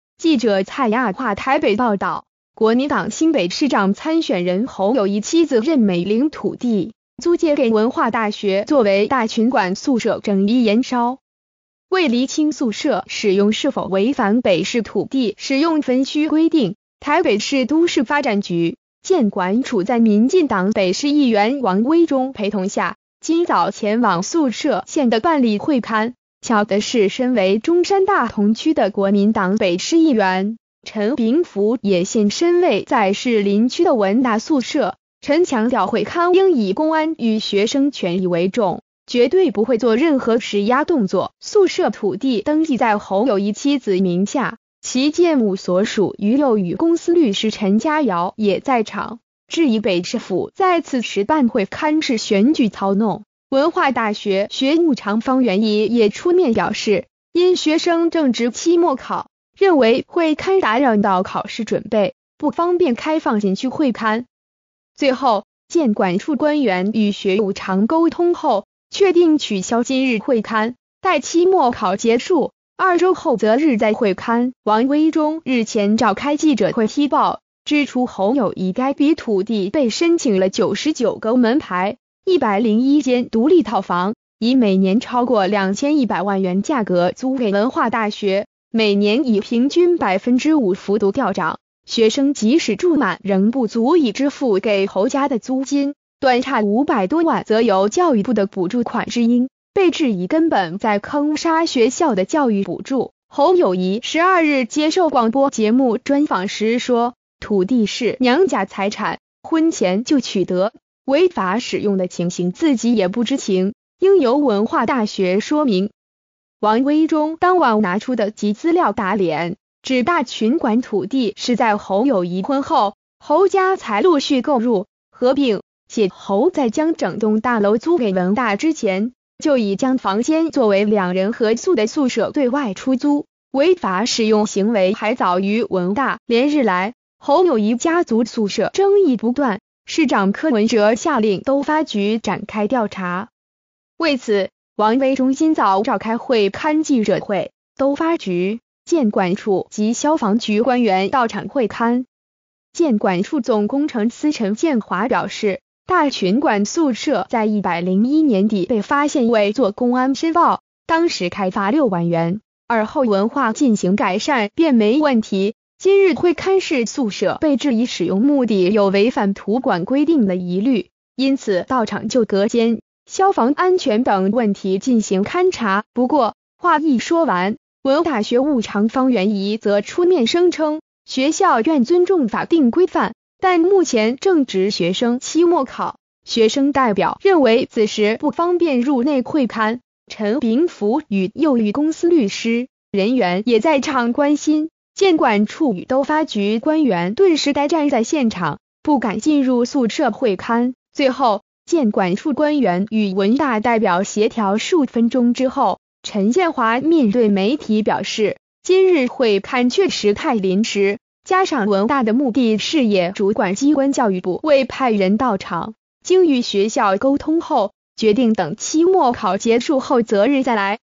藍營議員「跨區」到場關心 侯友宜宿舍爭議會勘取消
〔記者蔡亞樺／台北報導〕國民黨新北市長參選人侯友宜妻子任美鈴土地，租借給文化大學作為「大群館」宿舍爭議延燒。為釐清宿舍使用是否違反北市土地使用分區規定，台北市都市發展局、建管處在民進黨北市議員王威中陪同下，今早前往宿舍現地辦理會勘，巧的是身為中山大同區的國民黨北市議員陳炳甫也現身位在士林區的文大宿舍，陳強調會勘應以公安與學生權益為重，絕對不會做任何施壓動作。

發表評論評論如下。